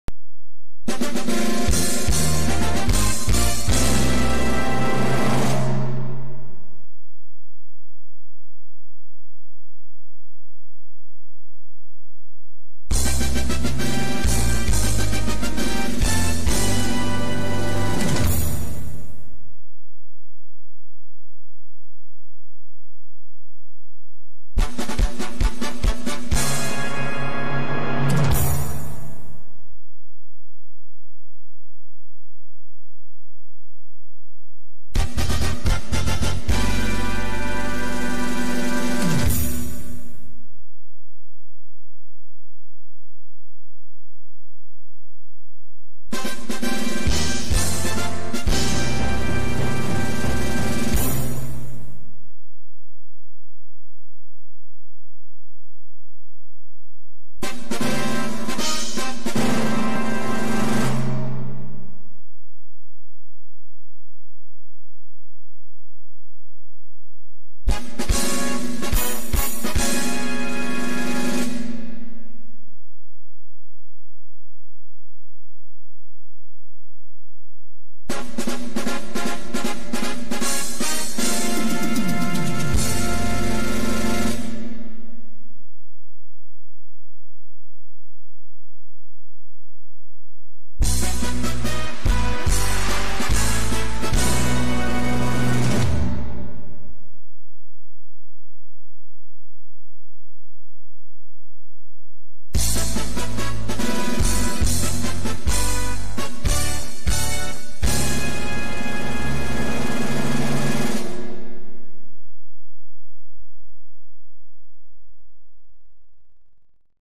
Fanfares 1-10 | Primera Reload: Smart Karaoke System
This is it guys! The Complete Fanfares for Primera Reload Smart K+ 2.0!

Audio Used: Reyna 3 Fanfares (DoReMi Channel)

Clips Used: 3D Dance BGV for Platinum Karaoke (KapitanzHD, Mark Lanao, Platinum Karaoke Powered by Android KTV)

This video is used for educational and entertainment purposes only.